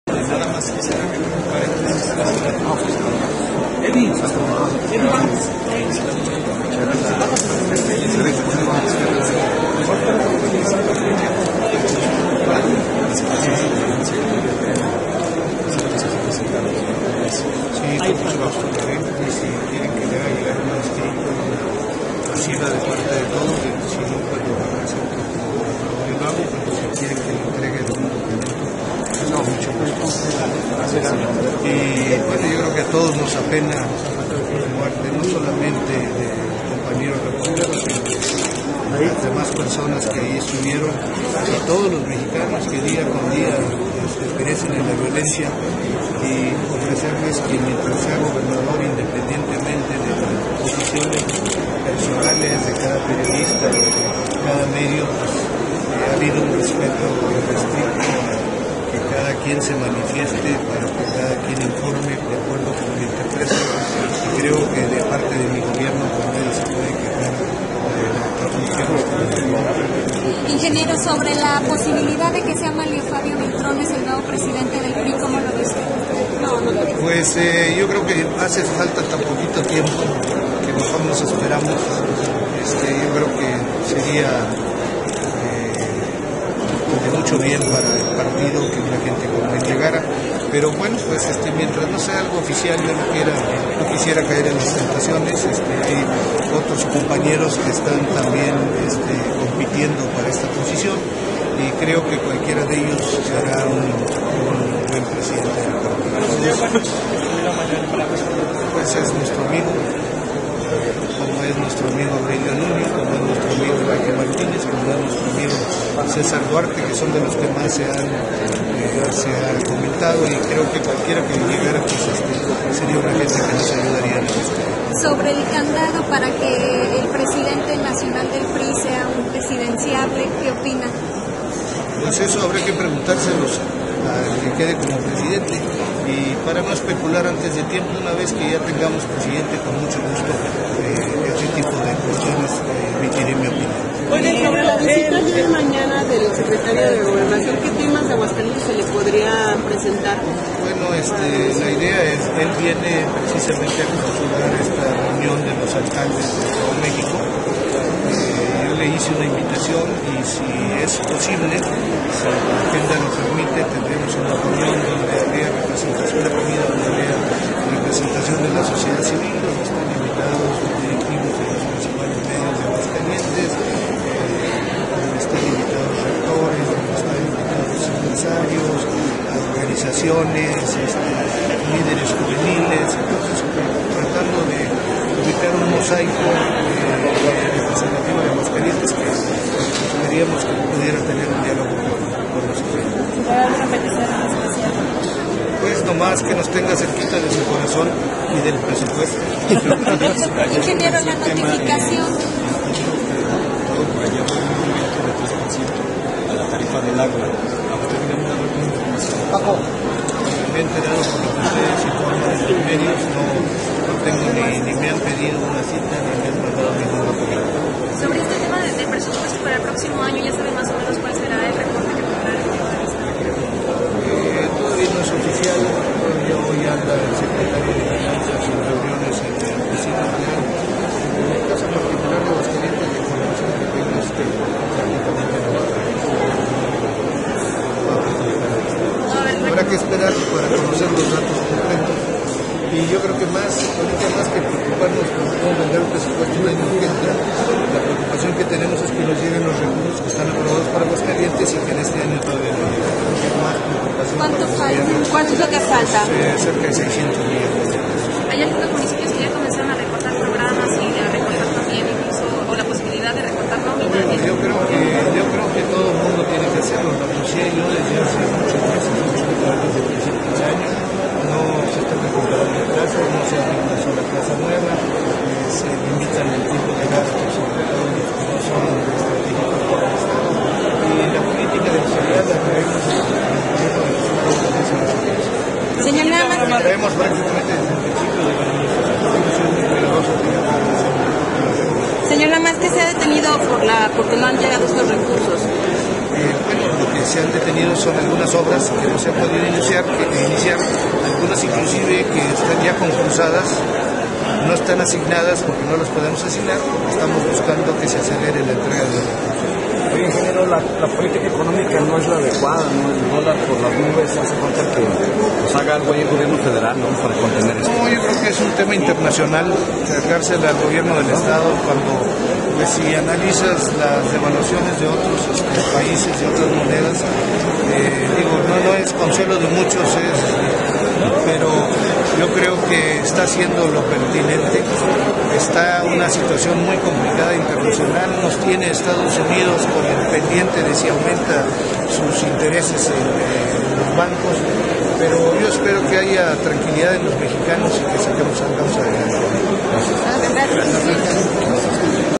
Mario Flavio Beltrones,Seria De Mucho Bien Para El Partido Una Persona Muy Entregada
ENTREVISTA AL GOBERNADOR DEL ESTADO,ING,CARLOS LOZANO DE LA TORRE,a todos nos apena la muerte del compañero reportero-CADA MEDIO HA HABIDO UN RESPETO QUE CADA QUIEN SE MANIFIESTE,QUE CADA QUIEN INFORME DE ACUERDO COMO LO INTERPRETE,creo que departe de mi gobierno nadie se puede quejar.